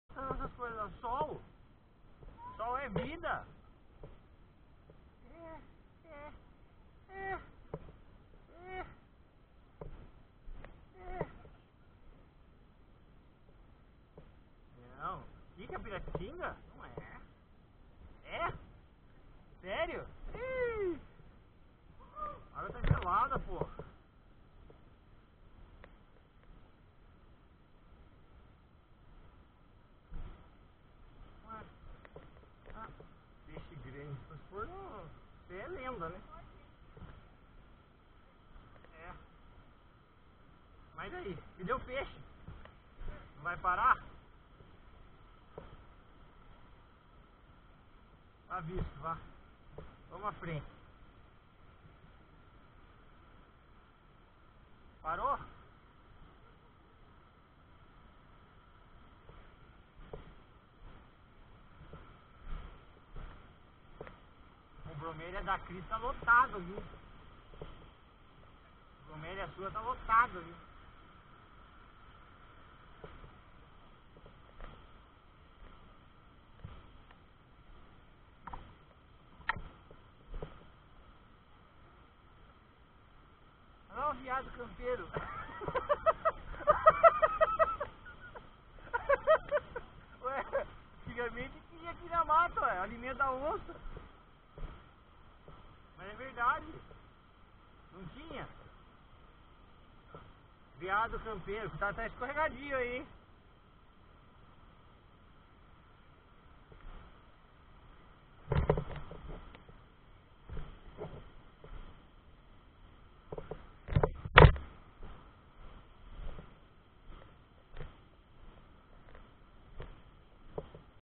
4k, ultra hd, Trilhas das cachoeiras, Trekking, Serra do Mar, 121
Trekking,  Núcleo Santa Virgina, Serra do Mar,  Trilha da Pirapitinga, cachoeiras do saltinho, salto grande, Ipiranga, Rio Ipiranga, Rio Paraibuna, trilhas, na Natureza Preservada da Serra do Mar, divisa de Ubatuba, São Luiz do Paraitinga. Com as atividades (rafting e trekking). Caminhada, com 13 amigos, 8,5 km, 600 m de altimetria, ...
Guia local Claudio.
Núcleo Santa Virgínia é uma Área de conservação localizado no interior do Parque Estadual da Serra do Mar.